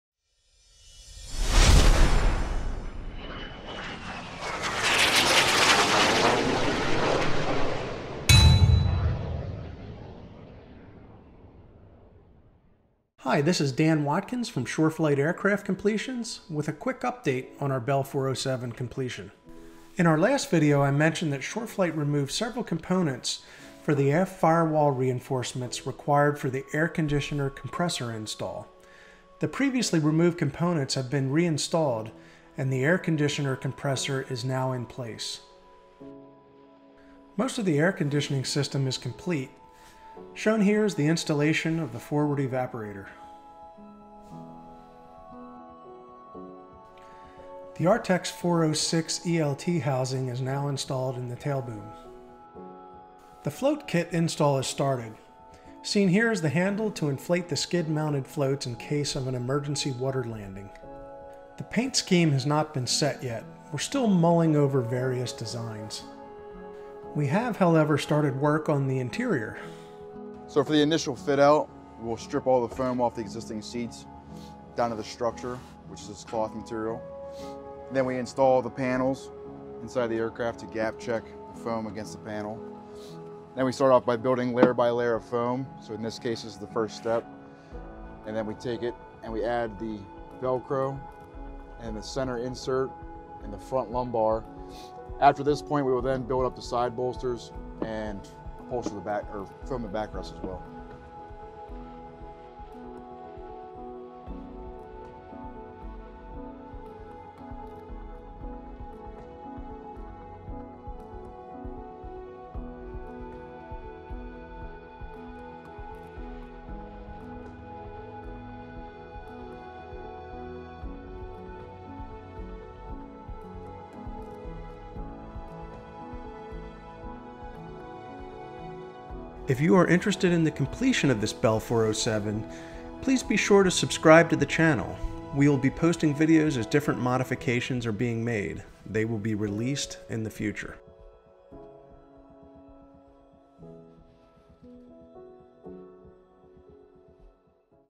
SureFlight Bell 407 GXi Completion Part 3 of 6
This video is Part 3 detailing the completion of a Bell 407 GXi by SureFlight Aircraft Completions at KMQS airport, PA.  Updates include air condition completion, ELT, the start of the float kit install and the interior seats.